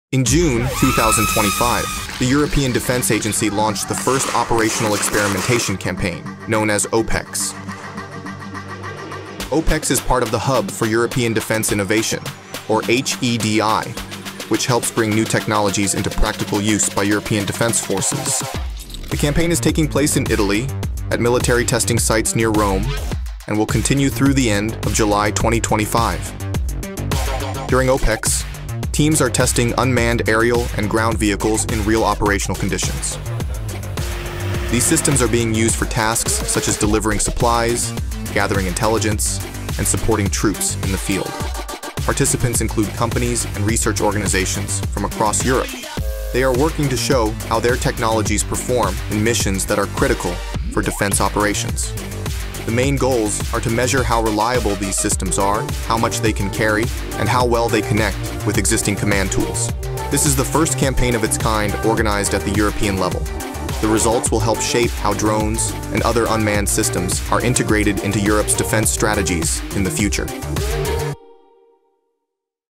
How Europe Is Testing Drones for Military | Inside the European Defence Agency’s OPEX Campaign 2025
In June 2025, the European Defence Agency launched the first Operational Experimentation Campaign, known as OPEX.

OPEX is part of the Hub for European Defence Innovation, or HEDI, which helps bring new technologies into practical use by European defence forces.

The campaign is taking place in Italy, at military testing sites near Rome, and will continue through the end of July 2025.

During OPEX, teams are testing unmanned aerial and ground vehicles in real operational conditions. These systems are being used for tasks such as delivering supplies, gathering intelligence, and supporting troops in the field.

Participants include companies and research organisations from across Europe. They are working to show how their technologies perform in missions that are critical for defence operations. The main goals are to measure how reliable these systems are, how much they can carry, and how well they connect with existing command tools.

This is the first campaign of its kind organised at the European level. The results will help shape how drones and other unmanned systems are integrated into Europe’s defence strategies in the future.